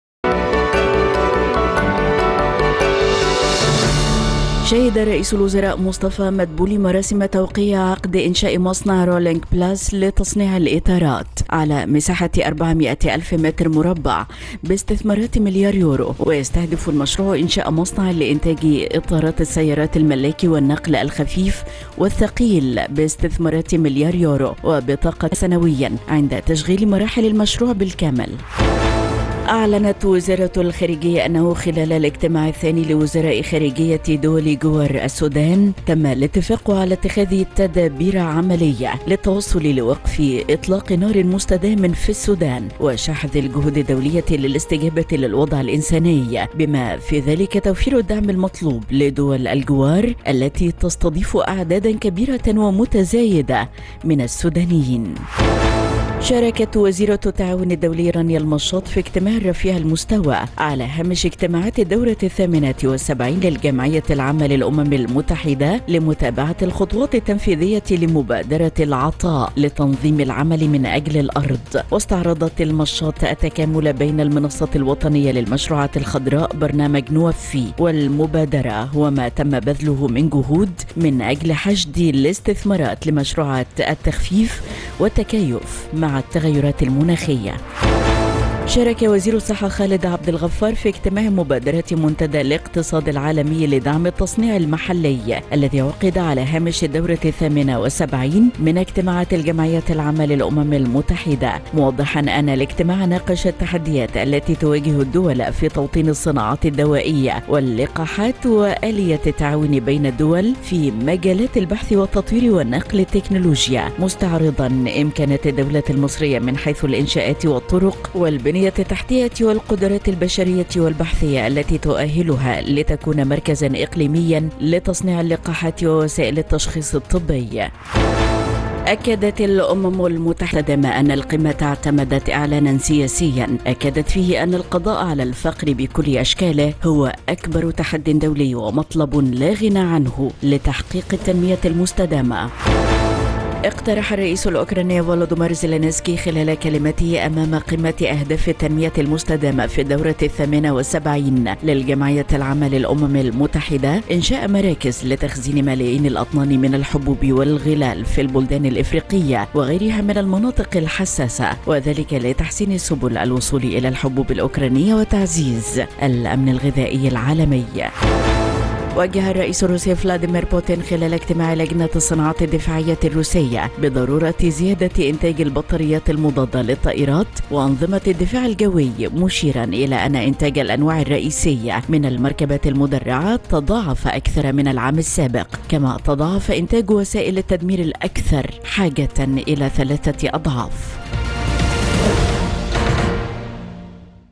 موجز أخبار الواحدة ظهرًا-وزير الصحة يستعرض قدرات مصر التي تؤهلها لتصبح مركزا إقليميا لتصنيع اللقاحات
أخبار أون